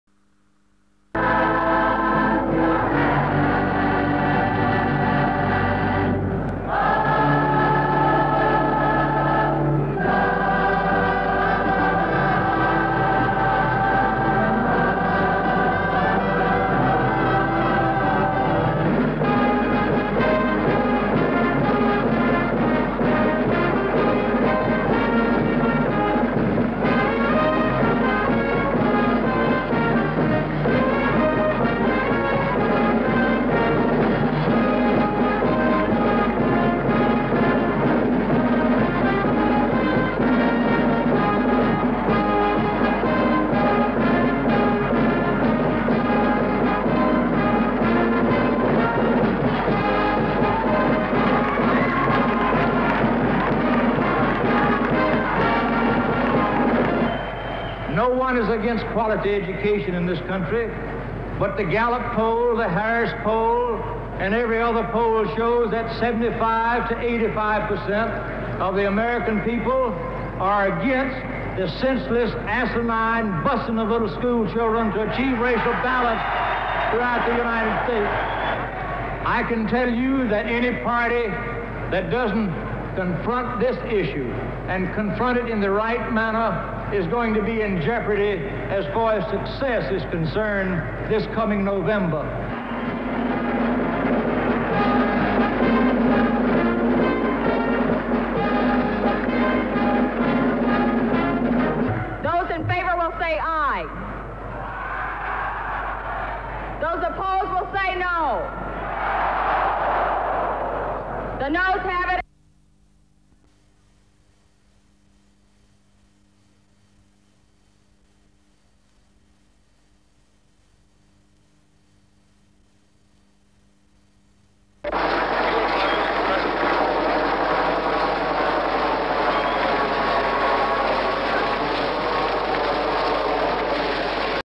SYND 12/07/72 SCENES IN MIAMI DEMOCRATIC CONVENTION
(12 Jul 1972) George Wallace addresses crowd at democratic convention in Miami.